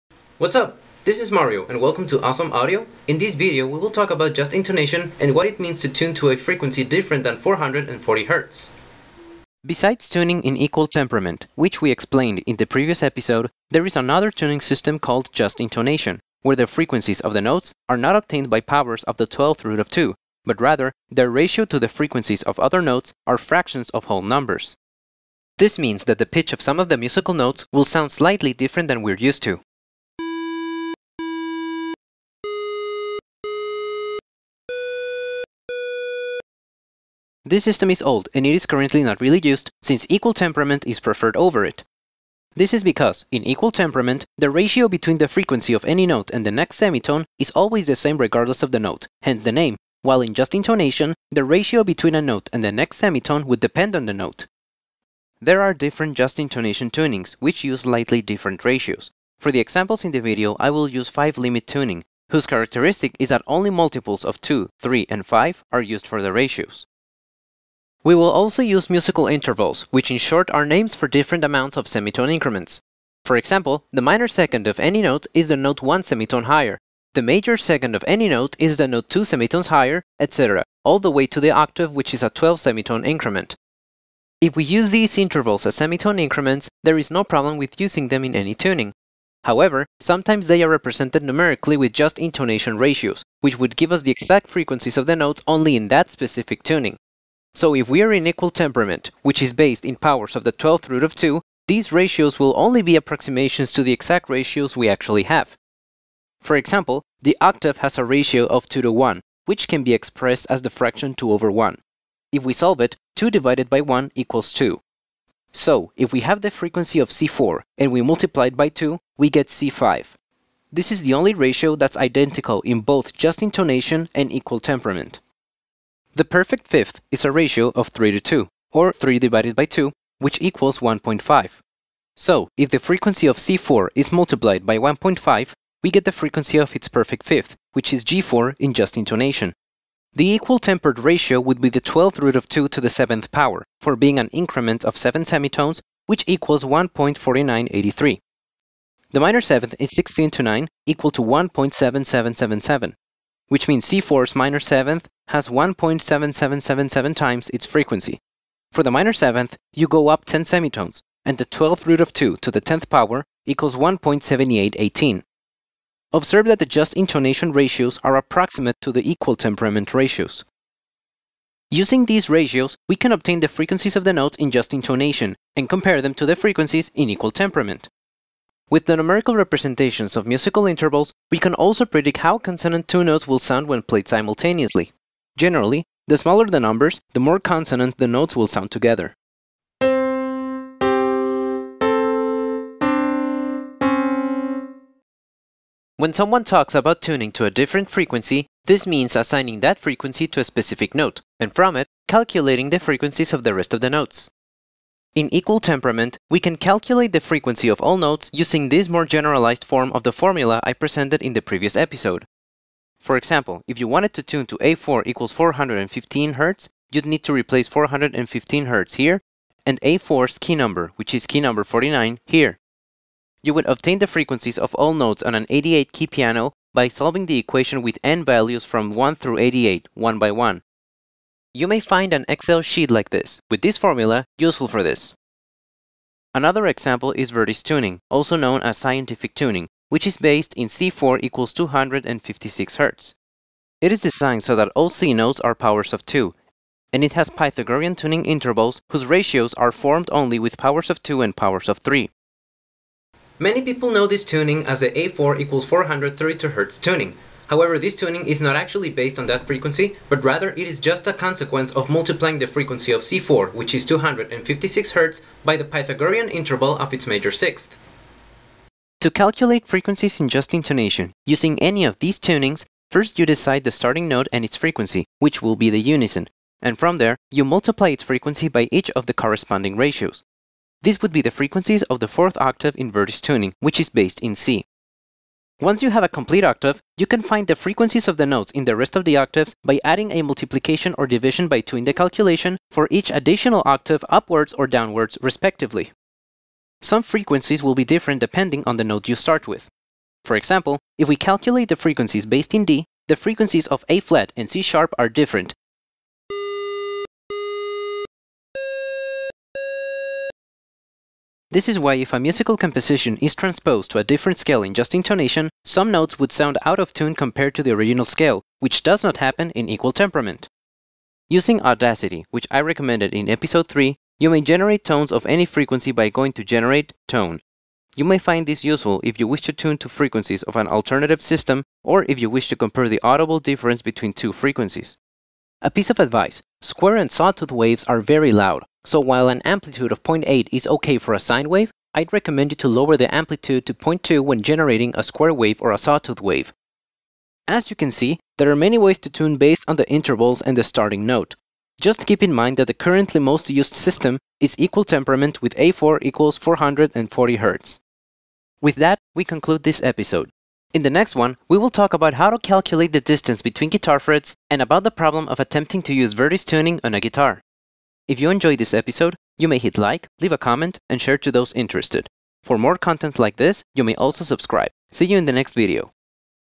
05 Just intonation and tuning to alternative frequencies [NEW VERSION IN THE DESCRIPTION]
Aside from the standard, in what other waves can frequencies be assigned to musical notes? In this video we will study alternative tunings, such as just intonation (limit 5 and pythagorean), their frequency intervals, how to calculate frequencies with this system, the problems in this system that don't exist in equal temperament, and how to tune a different frequency in equal temperament.

Excel formulal: =((2^(1/12))^(A2-n0)*f0 {replace n0 and f0 for key number and frequency of base note}